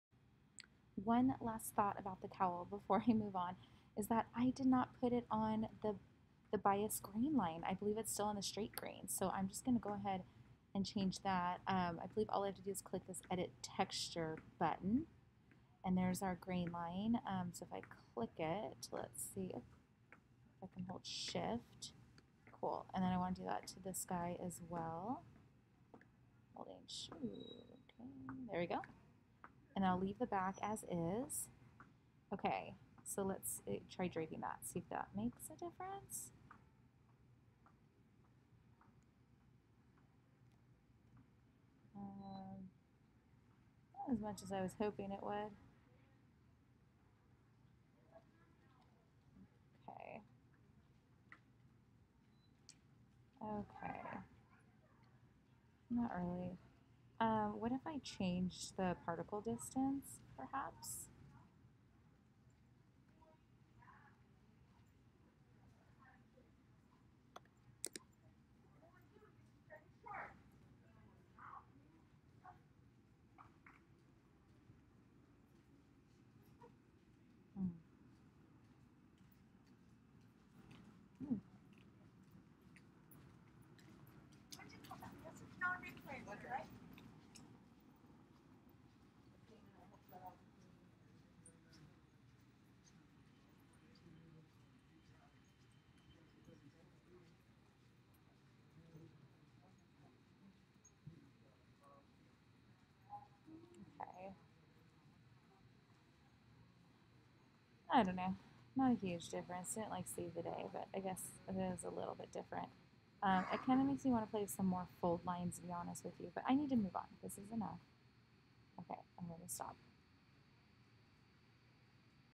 Will changing the grainline line direction help the drape of the cowl neckline in CLO3D
This video is for students enrolled in Pattern Drafting at Santa Ana College.

If you would like to follow along in the book, we use PATTERNMAKING FOR FASHION DESIGN, Joseph Armstrong, ISBN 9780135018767